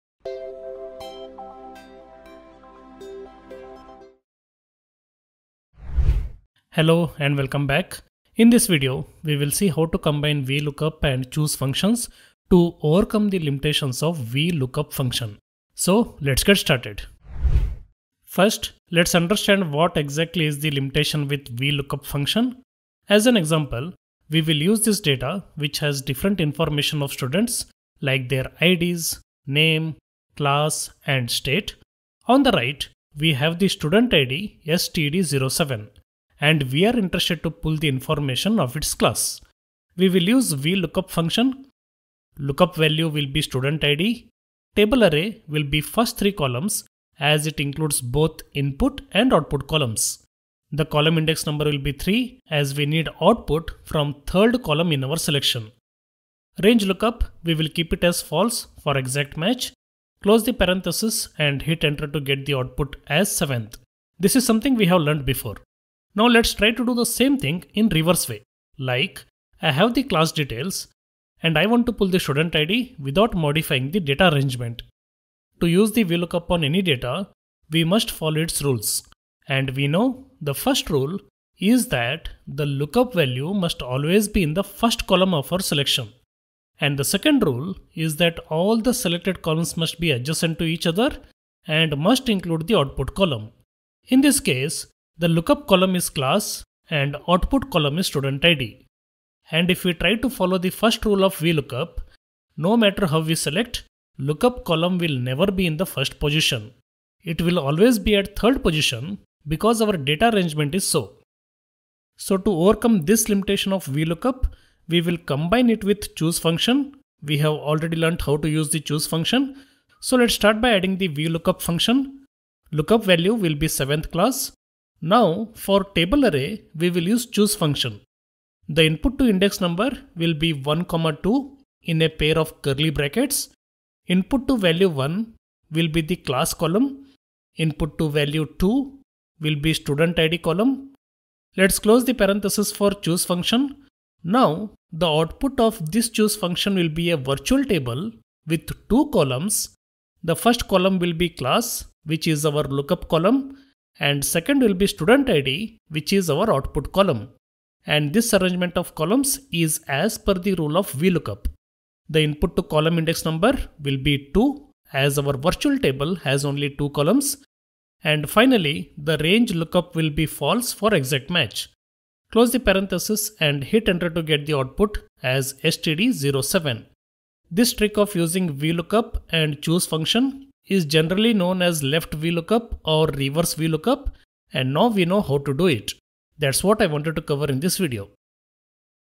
1.How to Use VLOOKUP with CHOOSE Function in Excel | Lookup from Left to Right Easily
Unlock the power of VLOOKUP and CHOOSE functions in Excel! In this tutorial, learn how to perform lookups even when your lookup column is to the right of the return column — a major limitation of VLOOKUP. This video shows you step-by-step how to combine VLOOKUP with CHOOSE to create flexible, powerful Excel formulas.

📌 Topics Covered:

Why VLOOKUP fails when lookup column is not the first

Syntax of VLOOKUP and CHOOSE

Using CHOOSE to reverse column order

Real-world example to understand the concept

Common mistakes and troubleshooting tips

Whether you're a beginner or intermediate Excel user, this tip will save you tons of time!